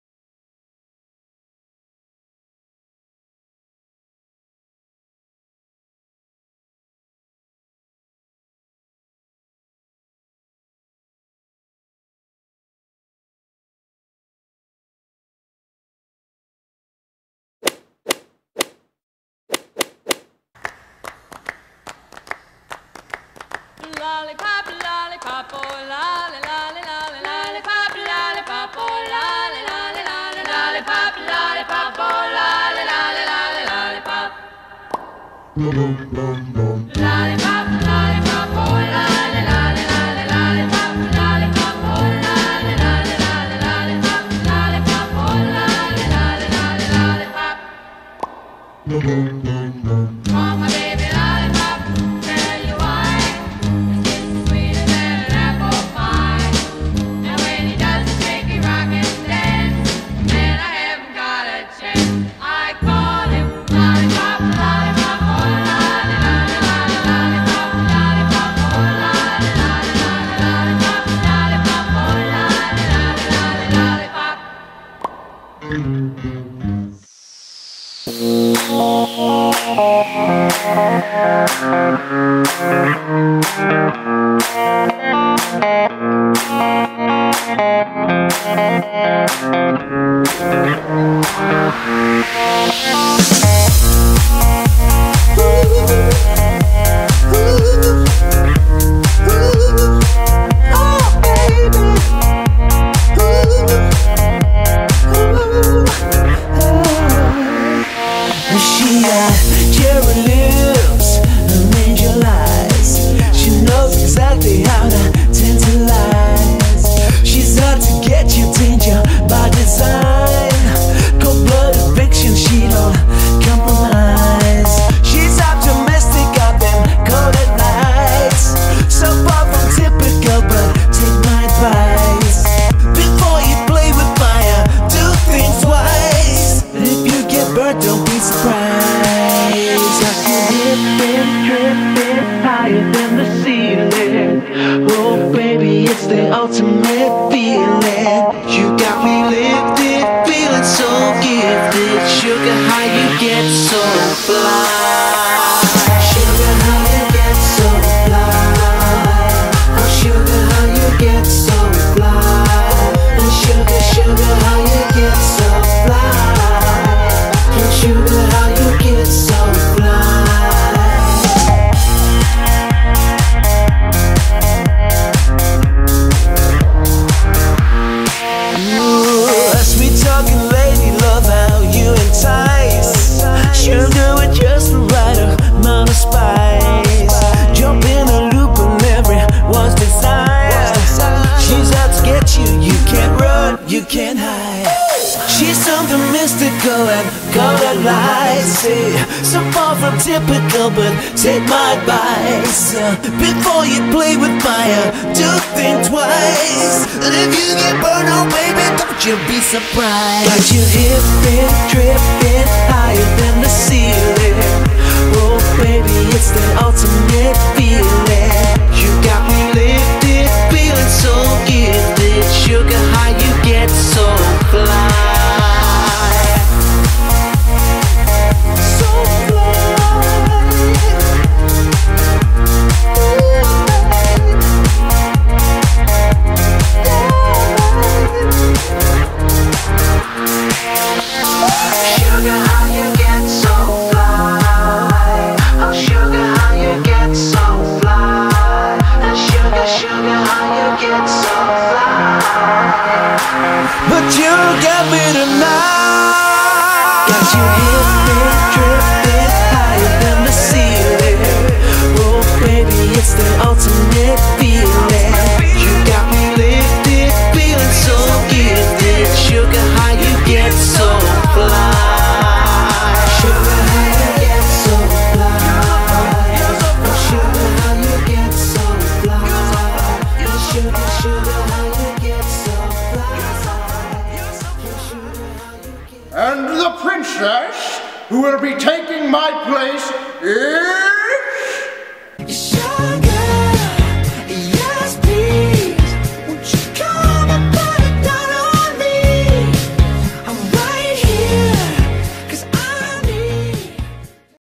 Homecoming Princess Video 2017
Here is the Homecoming Princess video for 2017! The theme this year is Candyland!